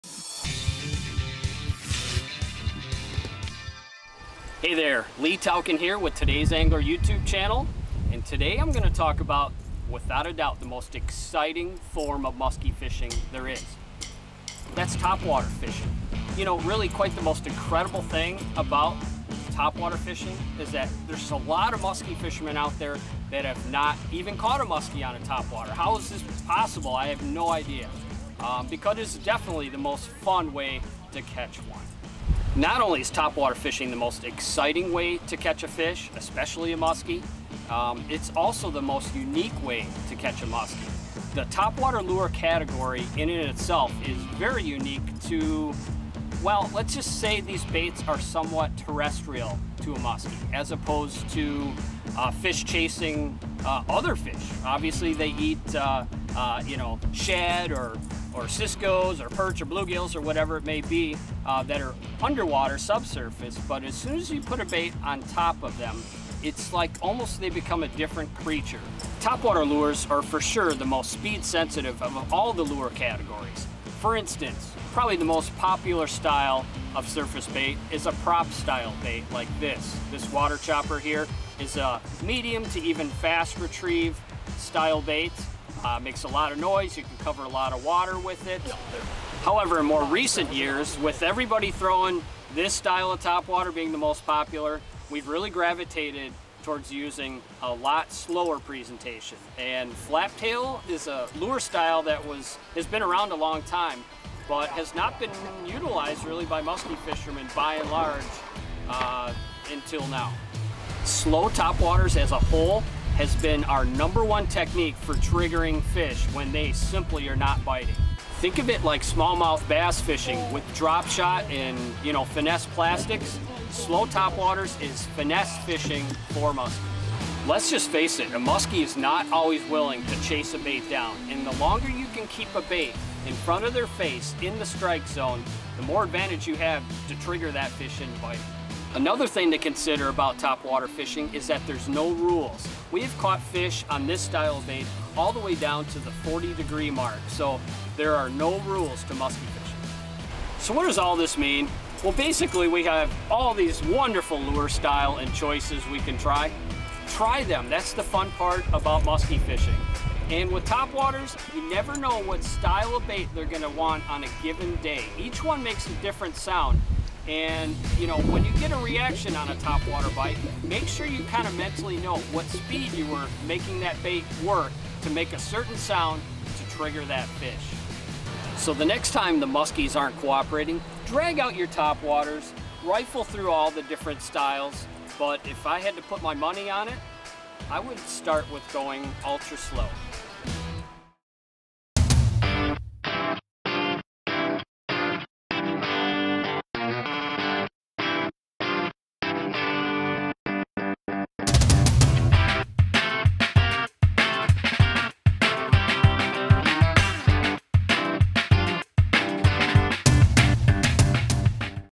Topwater Musky – Tips & Tactics (ft. Todays Angler)
Topwater musky fishing can provide some of the most exciting moments in freshwater fishing. Problem is, most folks don't understand just how many applications there are for topwater fishing, including in finesse situations and when the water is very cold. That's where the Todays Angler boys have surprised some folks in recent years.